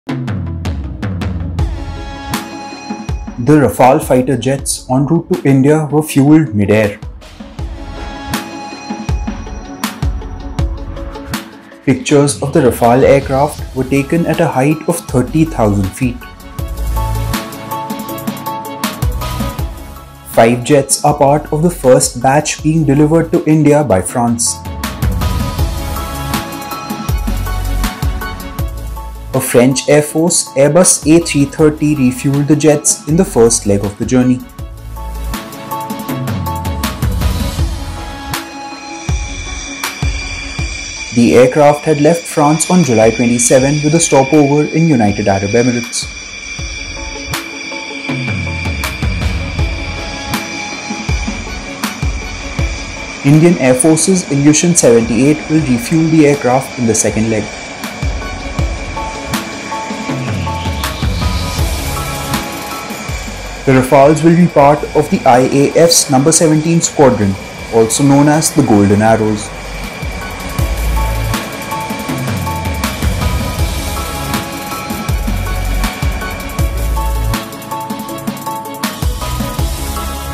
Watch: Rafale jets refuelled mid-air on their way from France to India
The 5 Rafale fighter aircraft en route to India were refuelled mid-air. Pictures of the refuelling exercise were taken at a height of 30,000 feet. The Indian Air Force 'appreciated the support' provided by the French Air Force for the Rafale journey. France used an Airbus A330 multi-role tanker transport to refuel the Rafale jets. The aircraft had left for India on July 27 with a stopover in the United Arab Emirates. While France provided fuel support in the first leg of the journey, IAF's Ilyushin-78s will take over for the second leg. This is the first batch of Rafales being delivered to India after a deal was finalised by Prime Minister Narendra Modi and his then French counterpart Francois Hollande in 2015. India has bought 36 Rafale jets for Rs 59,000 crore. They will be part of the IAF's No. 17 squadron, the 'Golden Arrows'.